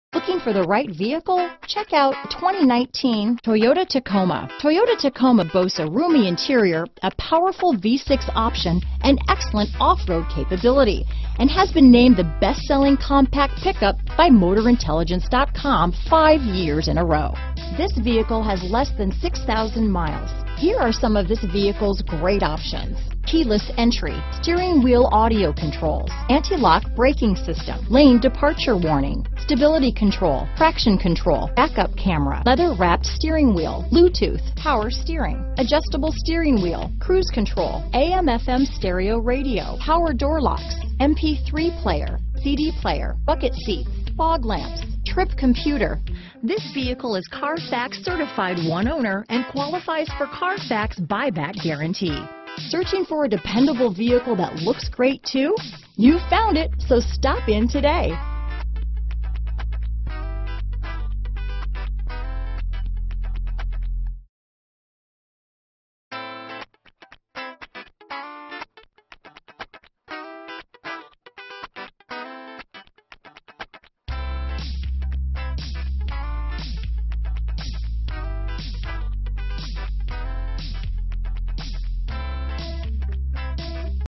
2019 Toyota Tacoma 2WD Tulsa, Broken Arrow, Joplin, Bixby, Owasso, OK T19127A
Gray Used 2019 Toyota Tacoma 2WD available in Tulsa, Oklahoma at Matthews Ford. Servicing the Broken Arrow, Joplin, Bixby, Owasso, OK area.
2019 Toyota Tacoma 2WD SR5 - Stock#: T19127A - VIN#: 5TFAX5GN9KX137074

Matthews Ford
1101 Southwest Expressway Drive

Cement 2019 Toyota Tacoma SR5 RWD 6-Speed Automatic 2.7L I4 DOHC 16V ALLOY WHEELS, APPEARANCE PACKAGE.Recent Arrival! Odometer is 3387 miles below market average!